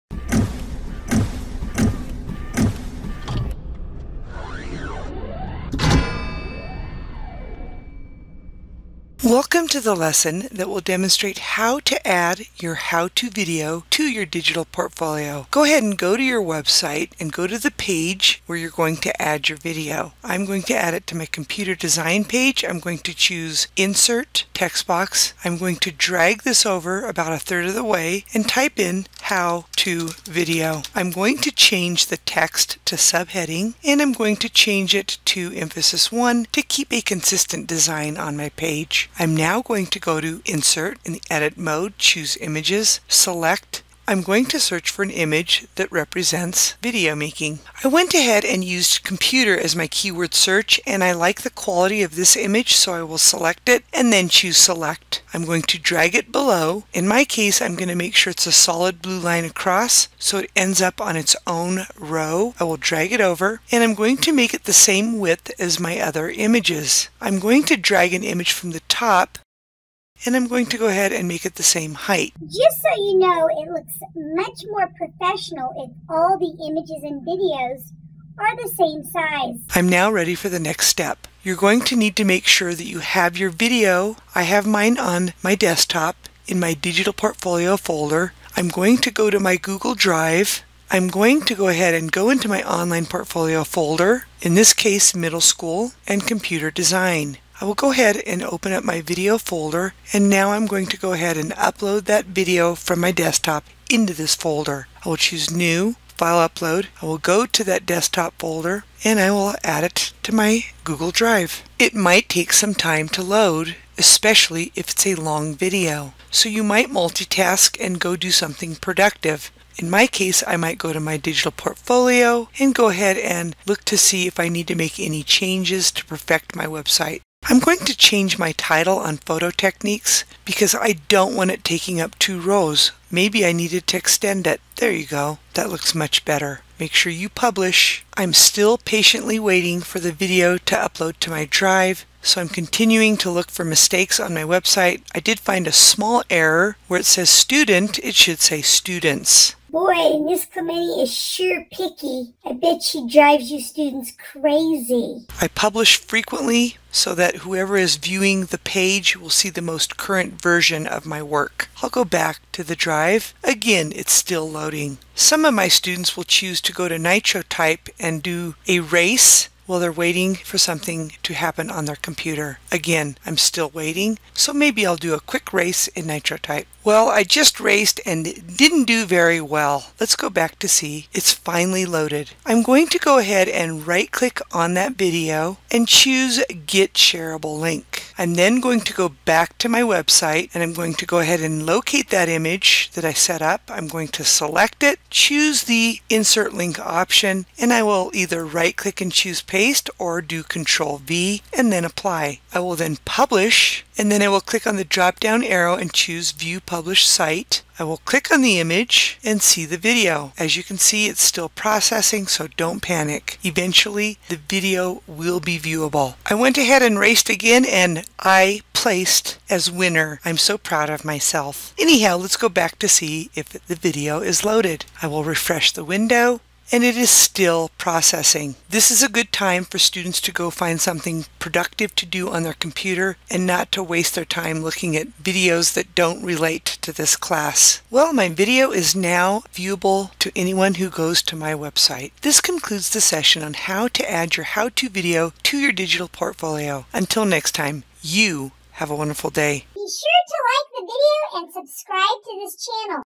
Student Portfolios: Adding Video to a Google Site
This video demonstrates how to add a video to a Google Site. In this case, it is a how to video being added to a student digital portfolio. Strategies are provided to students to multi task while waiting for video to load. I tried to add some humor to make it entertaining.